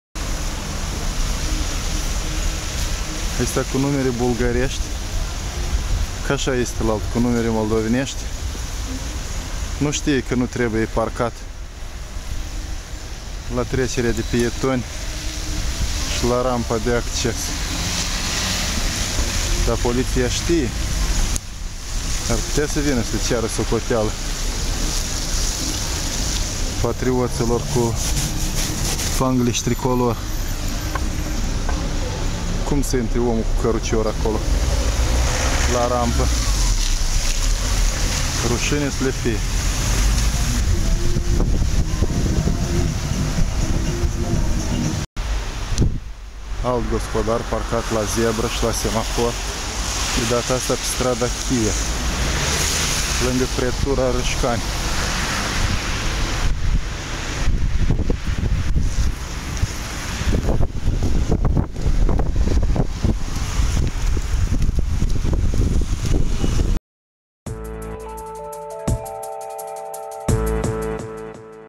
Au parcat pe trotuar, la zebră și rampa de acces - Curaj.MD
Doar trei mașini din sutele care încalcă legea și drepturile pietonilor, dar poliția și autoritățile nu fac nimic ca să oprească acest fenomen pandemic.